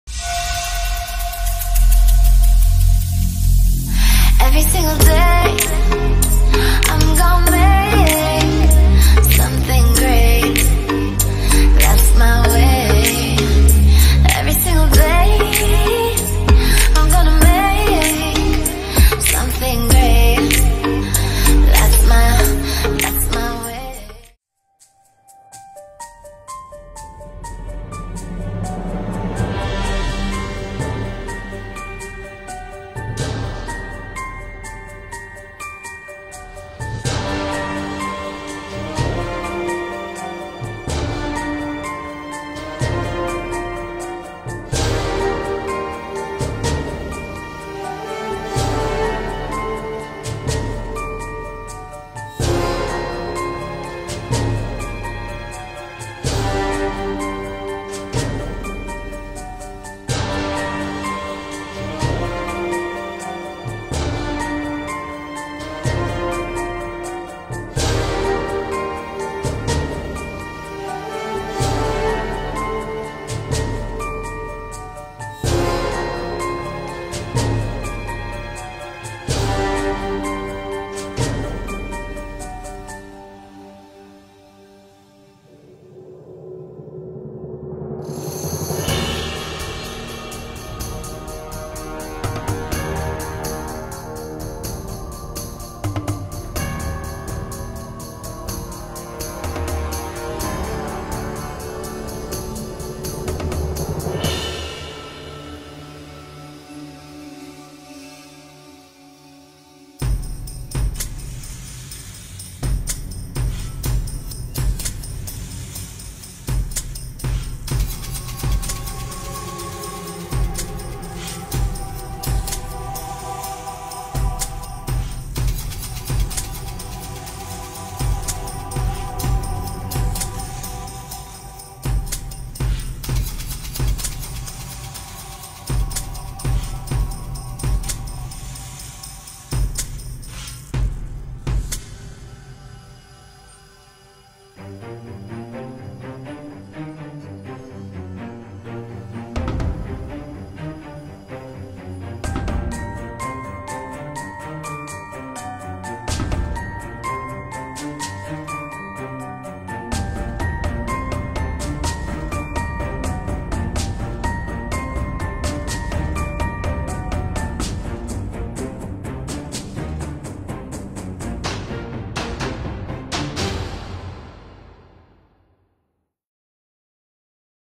FACES - Uno sguardo arrabbiato
Disegno in time lapse. Occhi arrabbiati... un esempio di espressione.